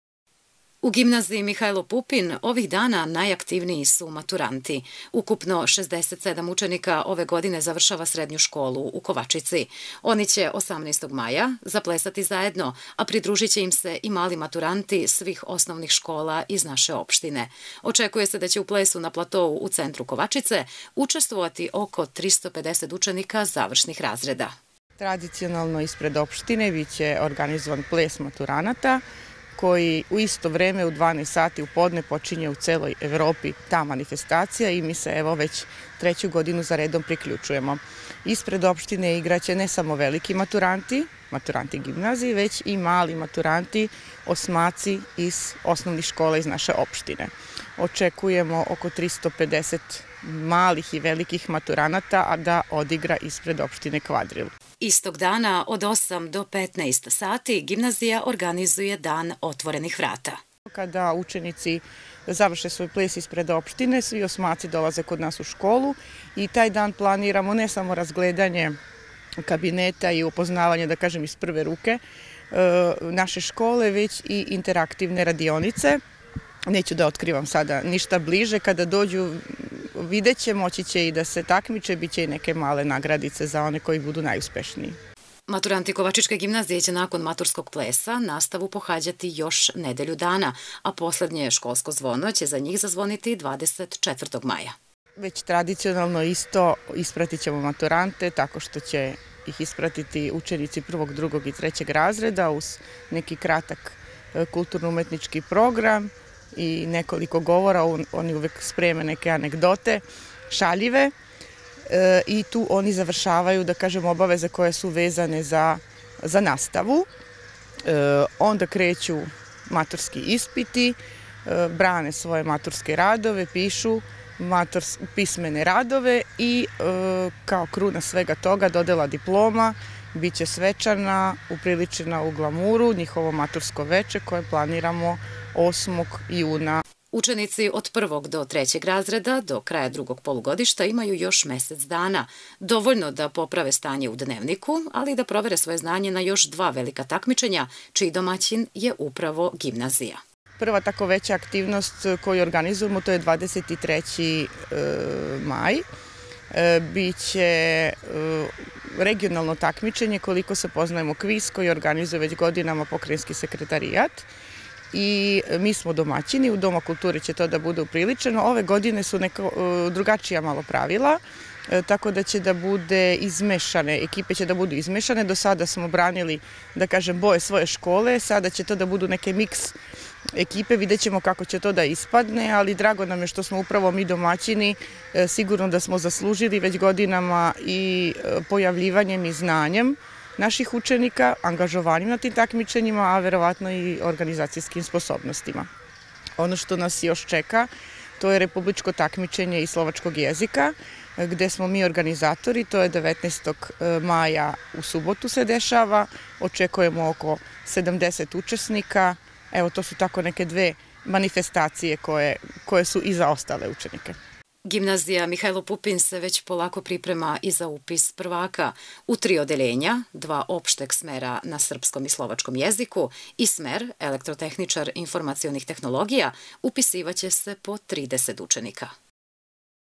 Ples malih i velikih maturanata 18. maja
Ples malih i velikih maturanata iz opštine Kovačica najavljen je za 18. maj tačno u podne. Istog dana Gimnazija “Mihajlo Pupin” organizuje Dan otvorenih vrata.